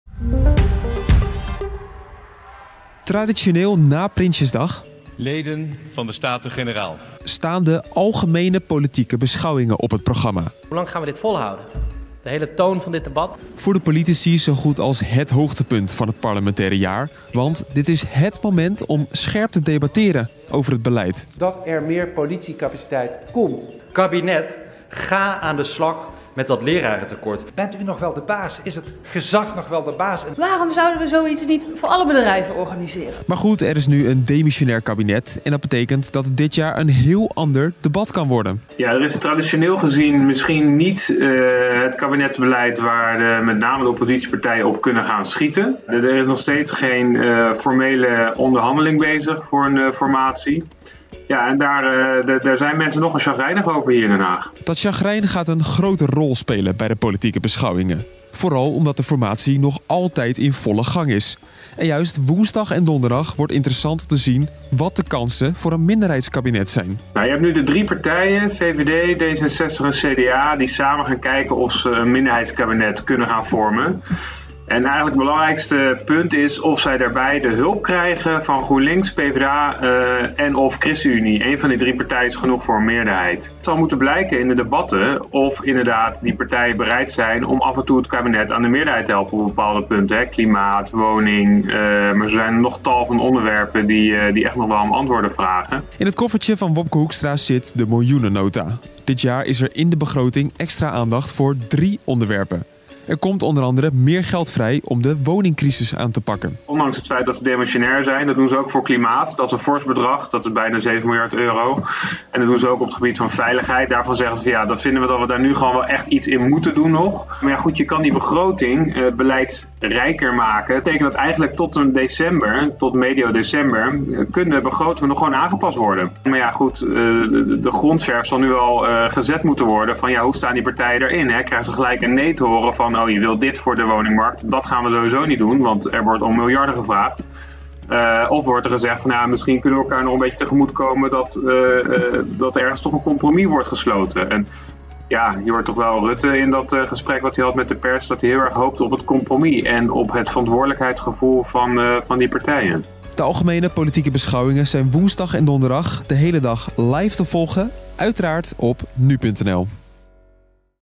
Waarom de Algemene Politieke Beschouwingen belangrijk zijn voor de formatie | NU.nl
Woensdag en donderdag staan de Algemene Politieke Beschouwingen op het programma. Voor politici zijn dat min of meer het hoogtepunt van het jaar, maar dit jaar speelt de formatie een grote rol in het debat. NU.nl-verslaggever Edo van der Goot legt in deze video uit waarom de APB belangrijk kunnen zijn voor de vorming van een nieuw kabinet.